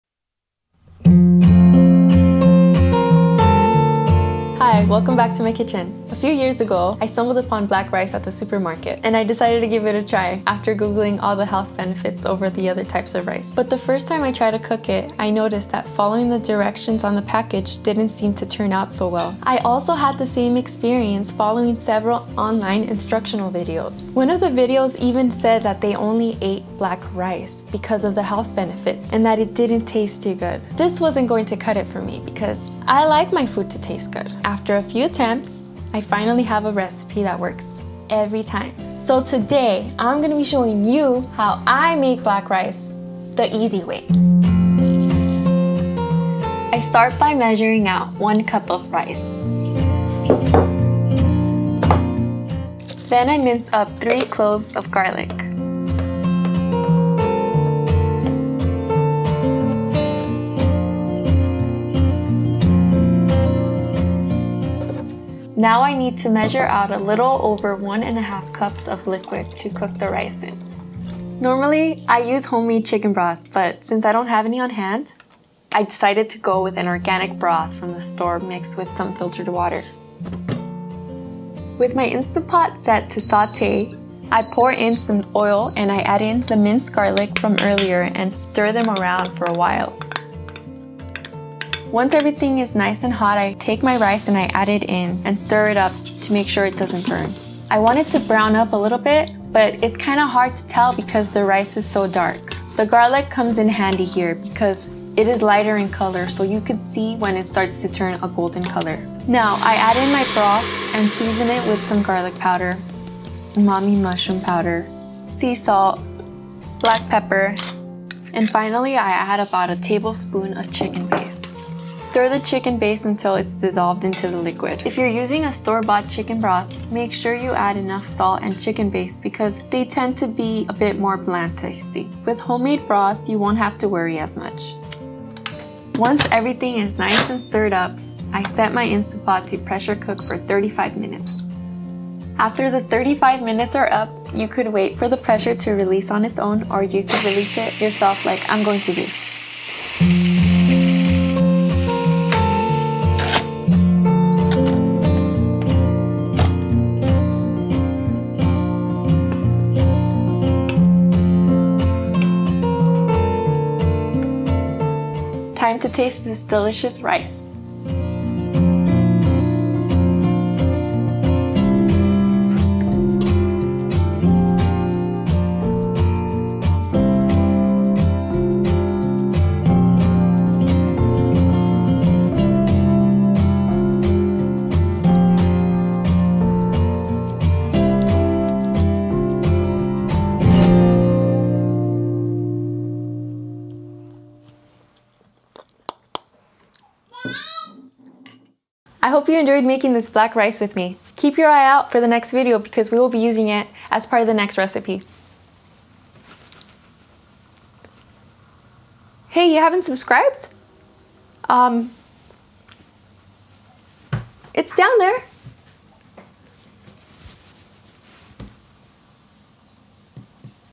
The Easiest And Most TASTY Black Rice Recipe | Black Rice Recipe Easy
Black Rice Recipe Easy!

In this video I show you how to make black rice the easy way. I noticed that a lot of people seem to think that black rice doesn't taste good and that it is only used as a healthy substitute for regular rice. Well, I'm here to tell you that you could make black rice, have it taste GREAT, and it will be EASY!!